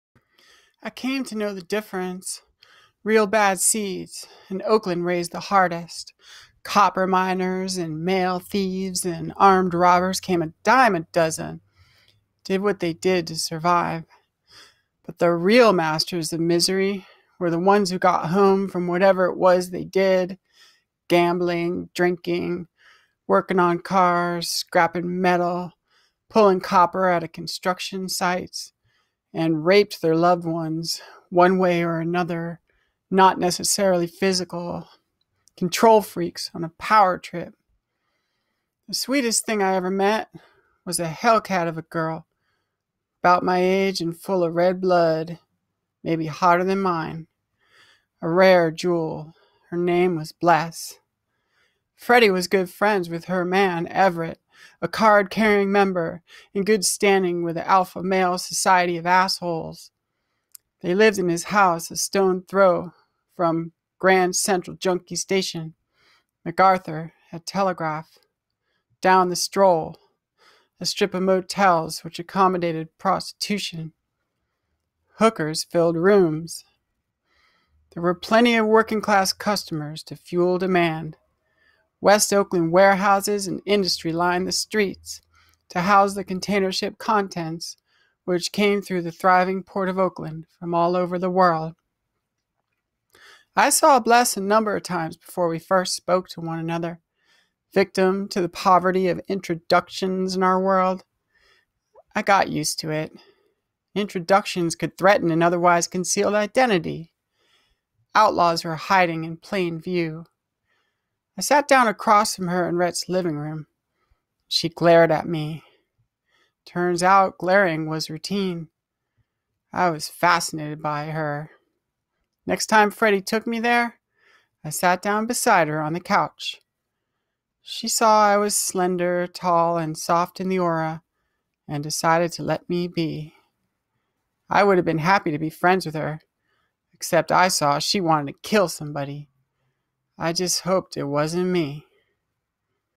GRAND THEFT LIFE 1.18.1
Created, written and read by Katya Mills from her novella, Grand Theft Life, published on Amazon and CreateSpace in 2015.